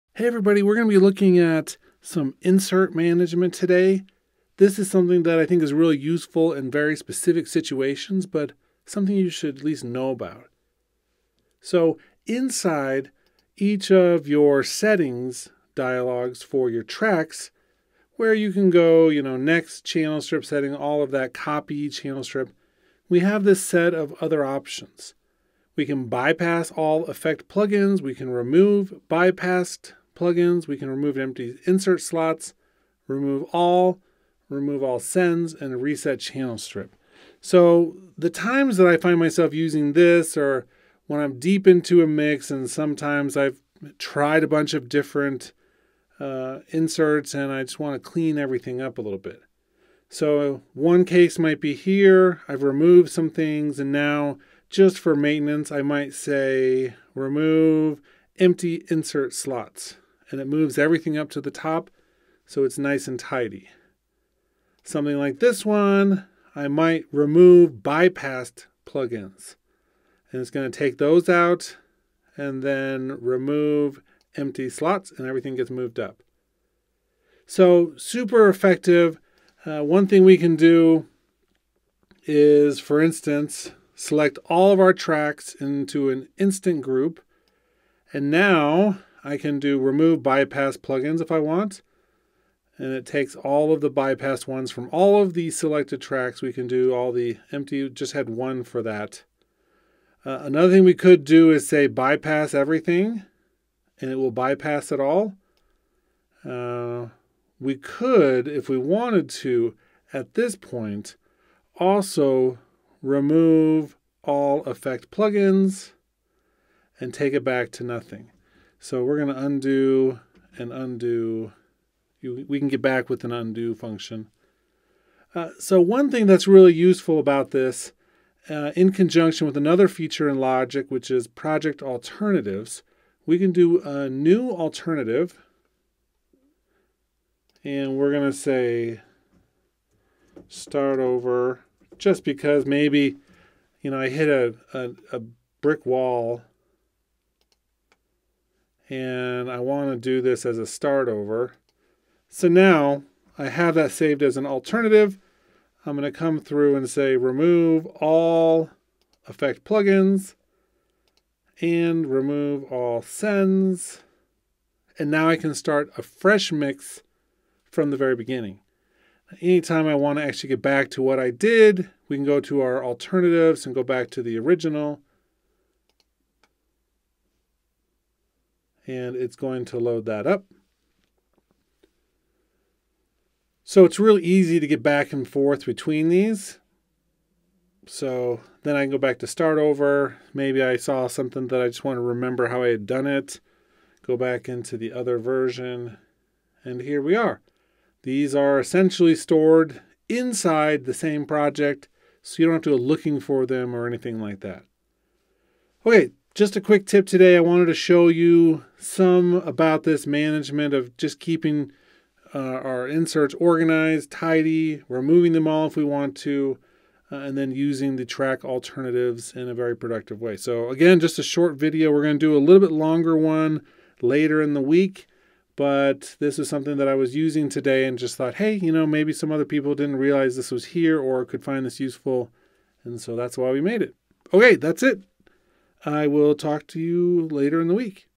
Insert Management | Logic Pro X
Here's an easy to watch, short tip for managing your inserts in the mixer. Enjoy!